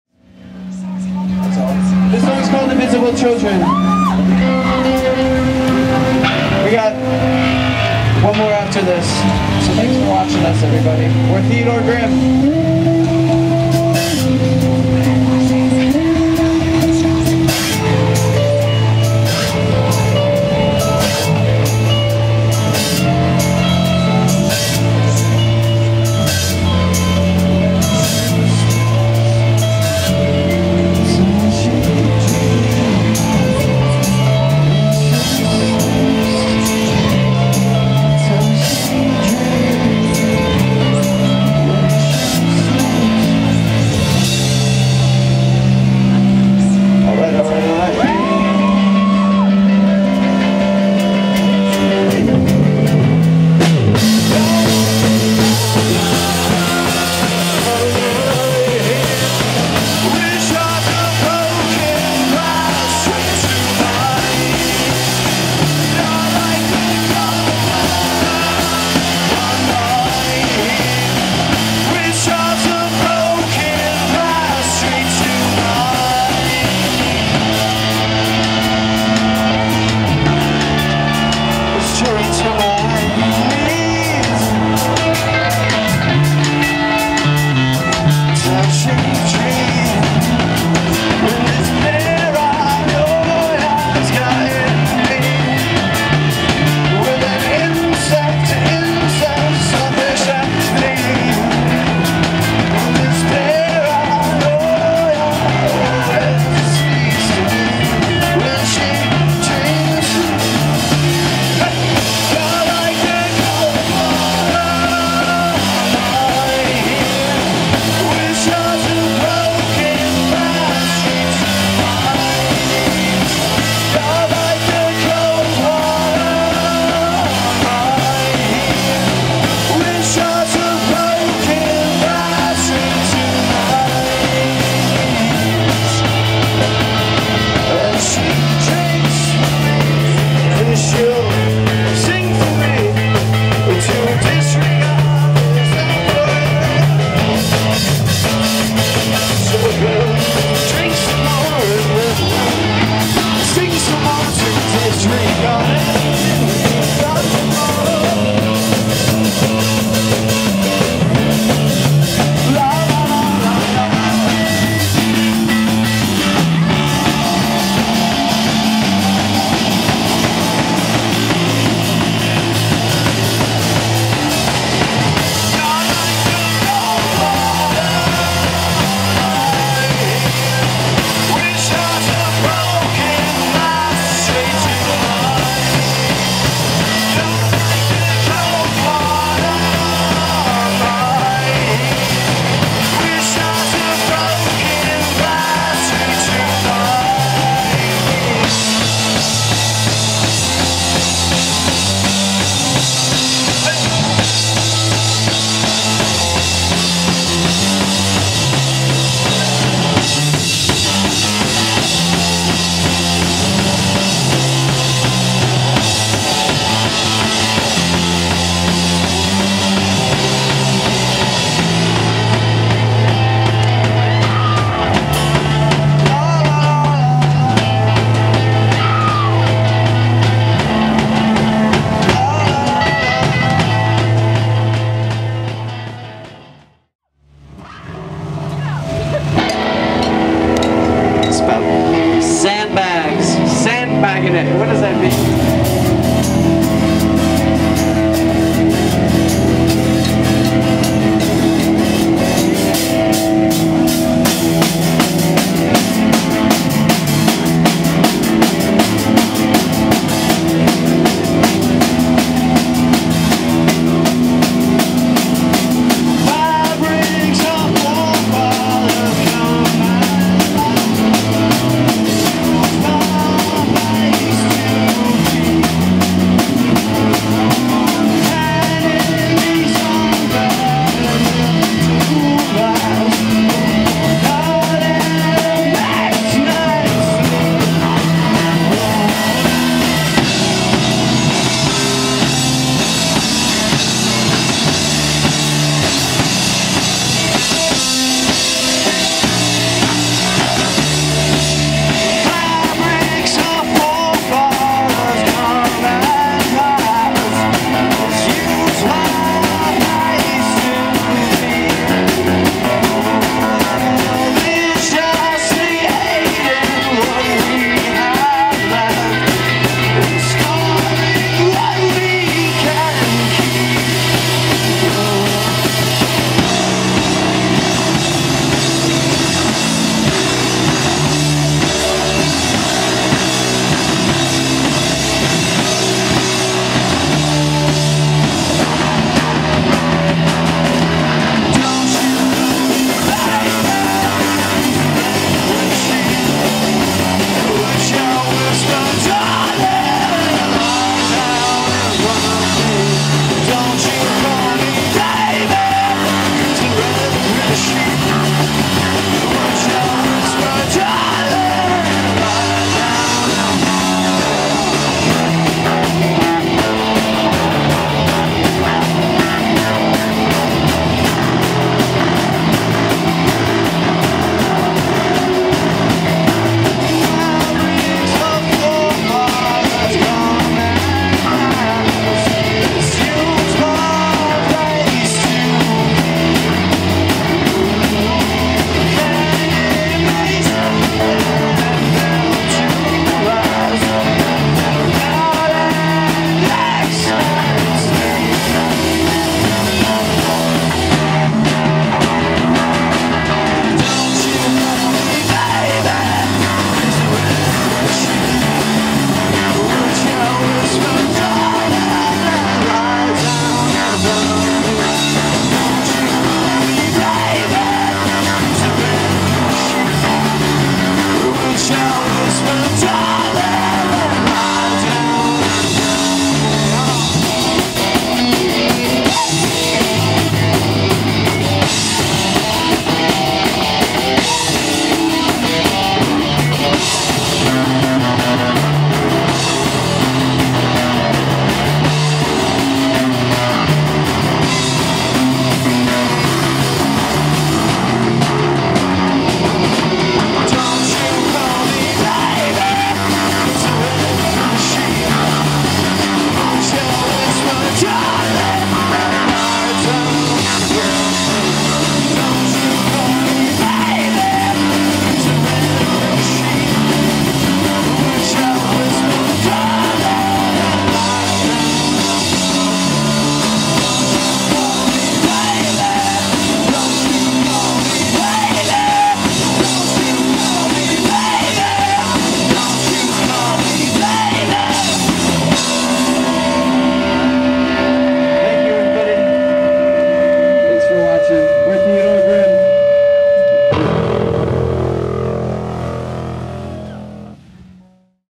Theodore Grimm - Warped Tour 09 "New Jersey"
Random Cat Films went to warped tour to film The STO Sideshow. Theodore Grimm at the STO Sideshow stage at Warped Tour 09 Oceanport, N.J.